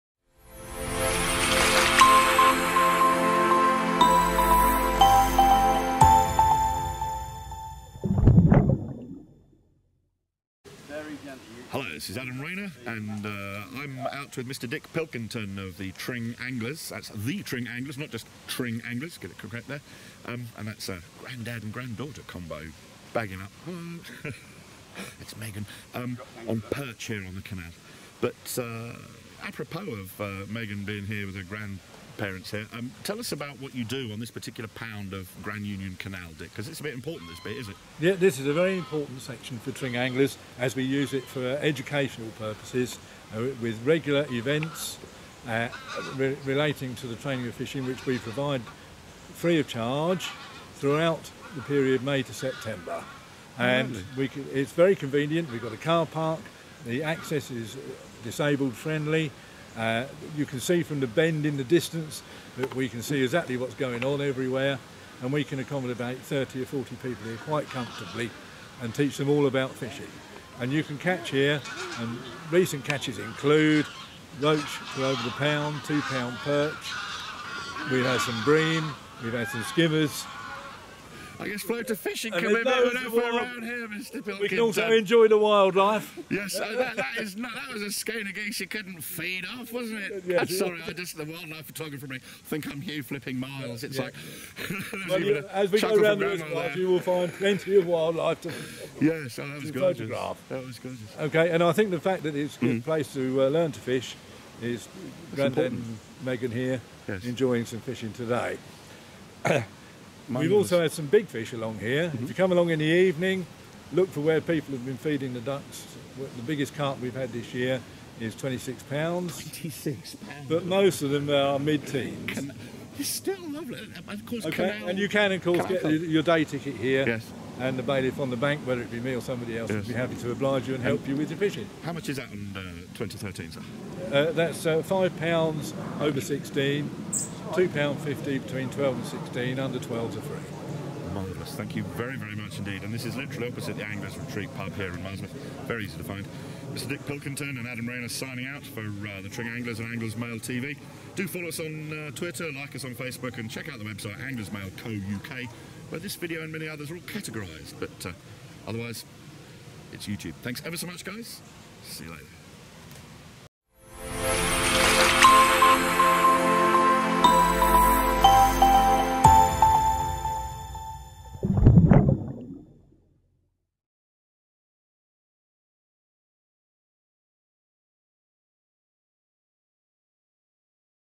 GRAND UNION CANAL, LOCKS 39/40, RESERVOIR POUND, MARSWORTH, BUCKs, ANGLERS MAIL TACTICAL BRIEFINGS
HP23 4LJ car park opposite the Anglers Retreat pub) for the Angler's Mail Where To Fish series.

Follow us @AnglersMail on Twitter